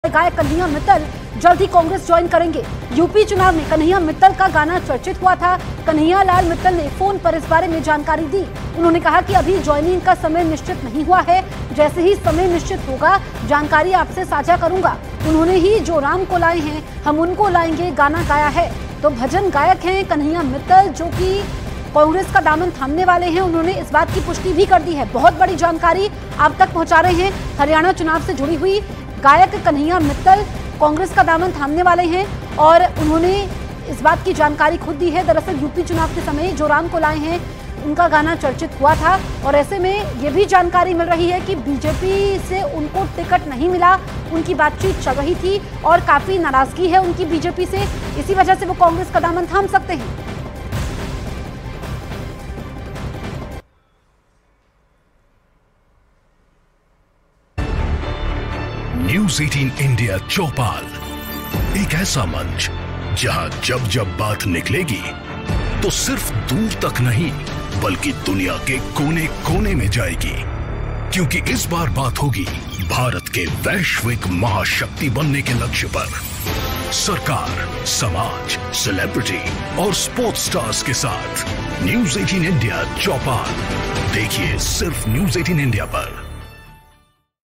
Haryana Election : कन्हैया लाल मित्तल ने फोन पर दी जानकारी, जल्द ही कांग्रेस में होंगे शामिल
Haryana Election:  सिंगर कन्हैया लाल मित्तल जल्द कांग्रेस में शामिल होंगे, कन्हैया लाल मित्तल ने फोन पर दी जानकारी, कहा अभी ज्वाइनिंग का समय निश्चित नहीं हुआ है, जैसे ही समय निश्चित होगा जानकारी साझा की जाएगी.